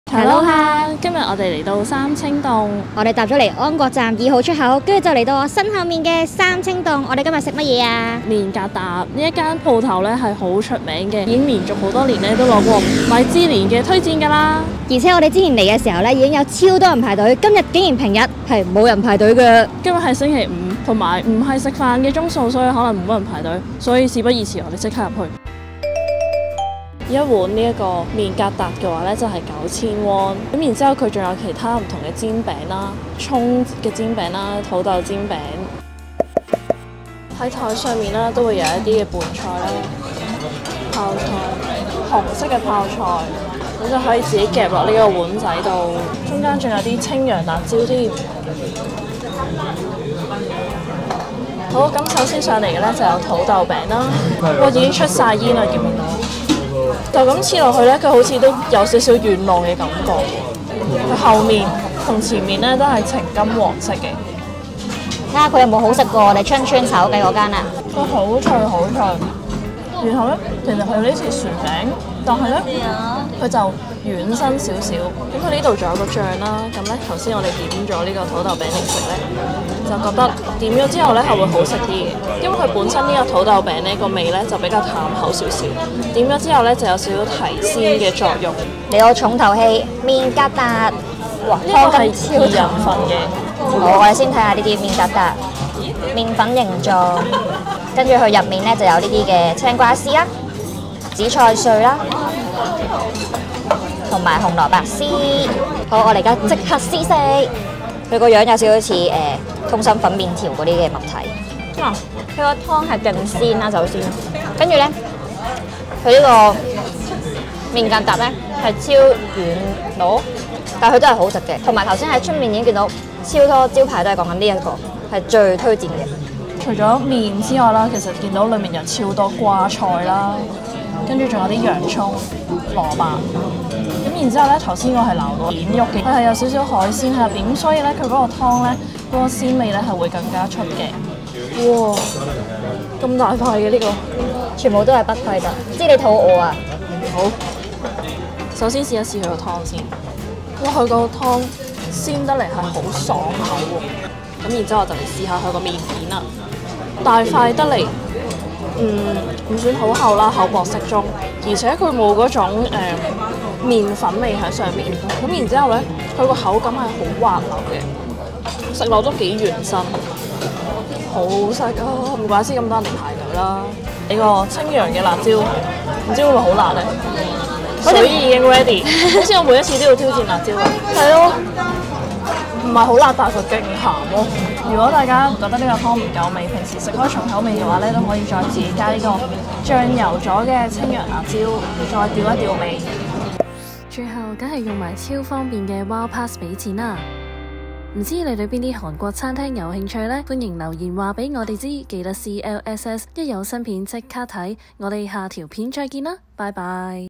【HungryKoreYan】三清洞米芝蓮麵疙瘩探店💮極鮮湯頭＋軟滑麵疙瘩🤤仲有薯仔煎餅味道如何😲？麵疙瘩
【HungryKoreYan】三清洞米芝蓮麵疙瘩探店💮
極鮮湯頭＋軟滑麵疙瘩🤤
仲有薯仔煎餅味道如何😲？
唔想一場嚟到但要大排長龍？
平日下午兩三點避開周末人流高峰！

地址：
三清洞麵疙瘩 (삼청동수제비)
首爾特別市鐘路區三清路101-1
서울특별시 종로구 삼청로 101-1

睇完記得ℂ𝕃𝕊𝕊！
留ℂ𝕠𝕞𝕞𝕖𝕟𝕥、畀𝕃𝕚𝕜𝕖、同人𝕊𝕙𝕒𝕣𝕖、齊齊𝕊𝕦𝕓𝕤𝕔𝕣𝕚𝕓𝕖！

【This video is created by 𝕁𝕆𝕂𝔼𝟚𝕐𝔸ℕ】